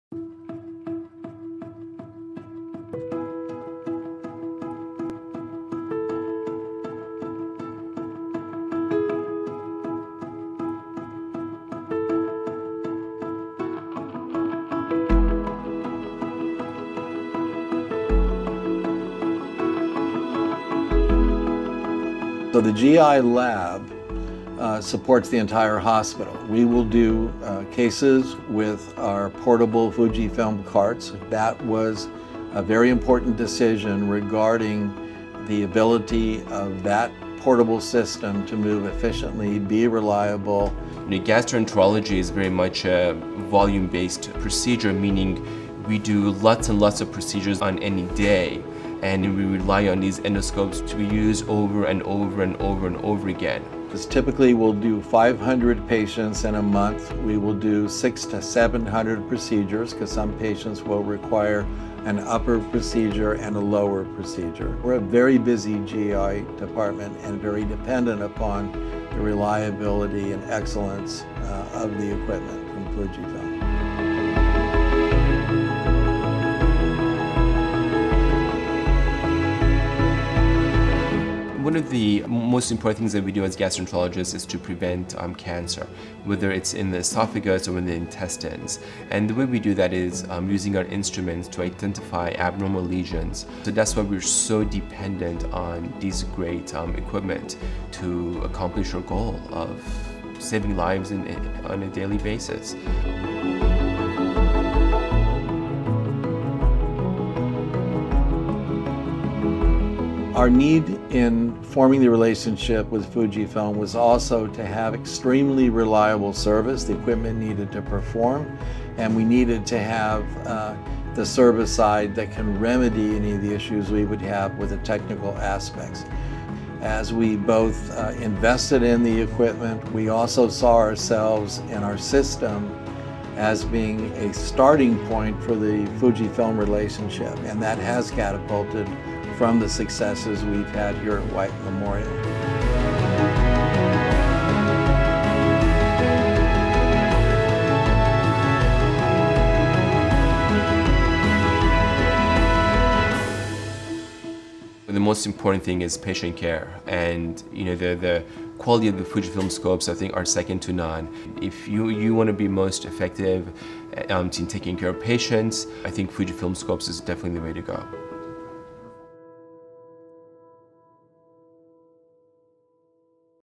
Fujifilm Healthcare - Endoscopy
With approximately 600-700 upper and lower procedures a month, Adventist Health White Memorial turns to Fujifilm’s endoscopy technology for state-of-the-art image quality and a wide range of endoscopes with integrated video carts for a variety of uses.